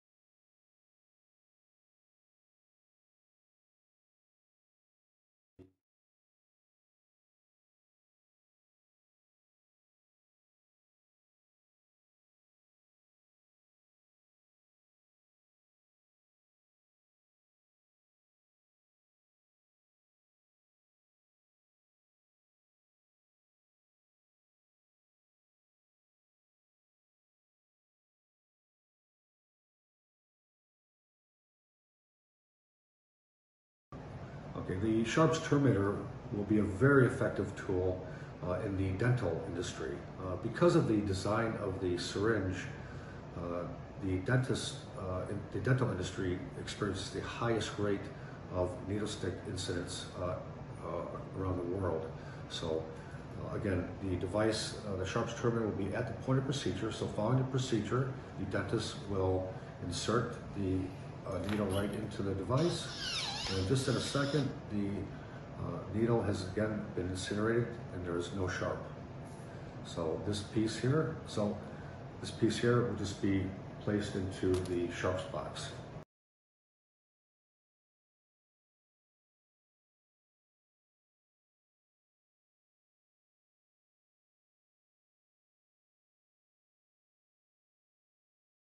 Work Smart, Don't get Pricked.
The Sharps Terminator will help Dentist, Dental Assistants, Student and researchers reduce needle stick injuries and potential infectious disease.